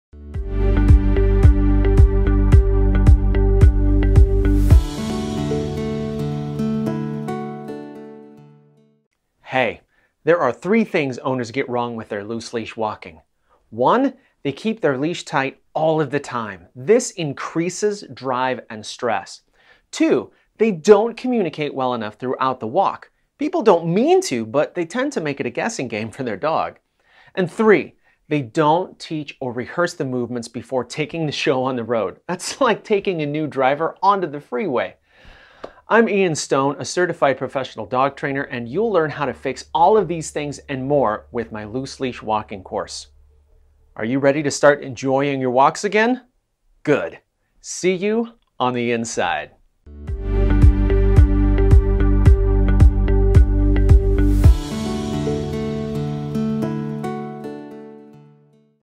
Loose Leash Dog Walking Course with Ian Stone
Dog training is made simple in our Loose Leash Dog Walking course. Learn effective steps for how to train your dog to walk gently on a leash, in order to get the bonding time you both need. Professional animal trainer Ian Stone will walk you through every module, with methods that have been proven effective at his dog training school in New York.

✋ Enjoy a happier, healthier relationship with your dogs. Start dog leash training with Ian Stone now.